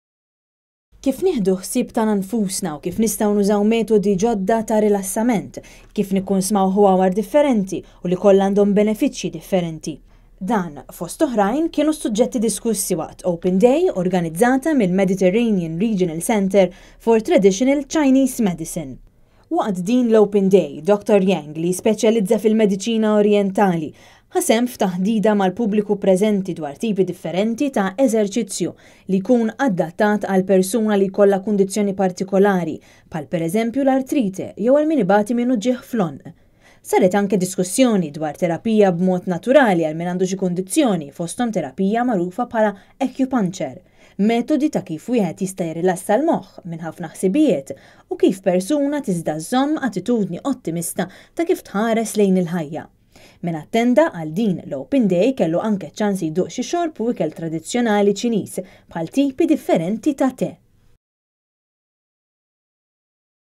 OPEN DAY DWAR KIF NIEĦDU ĦSIEB TAGĦNA NFUSNA
Kif nieħdu ħsieb tagħna nfusna u kif nistgħu nużaw medtodi ġodda ta' rilassament, kif nikkunsmaw ħwawar differenti u li kollha għandhom benefiċċji differenti, kien suġġetti diskussi waqt Open Day organizzata mill-Mediterranean Regional Centre for Traditional Chinese Medicine.